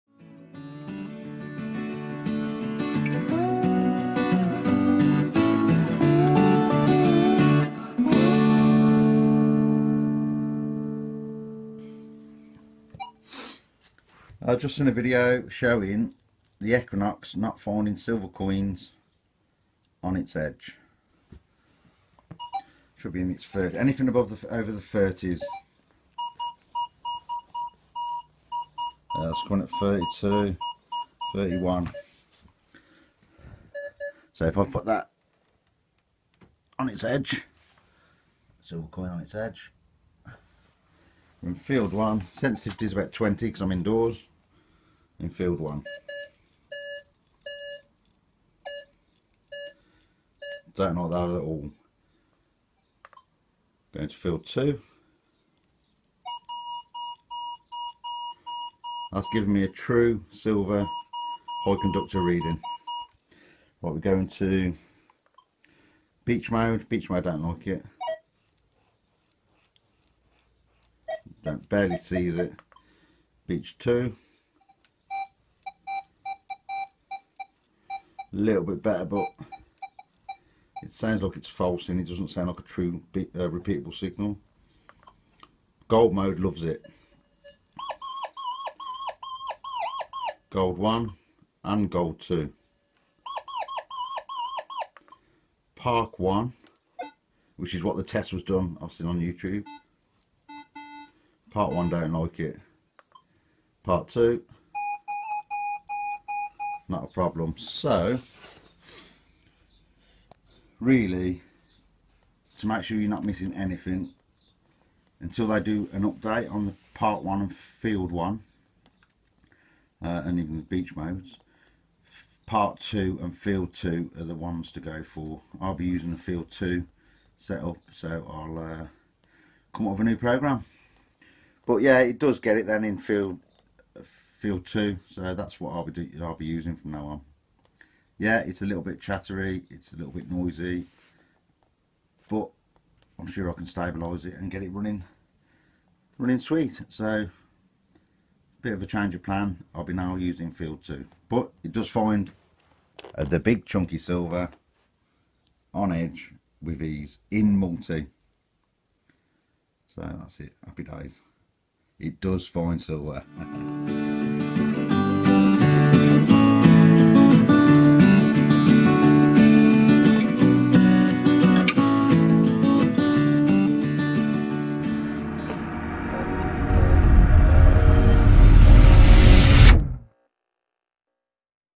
Minelab equinox, how to get silver coins on edge, metal detecting
An easy fix to not even miss silver coins on edge...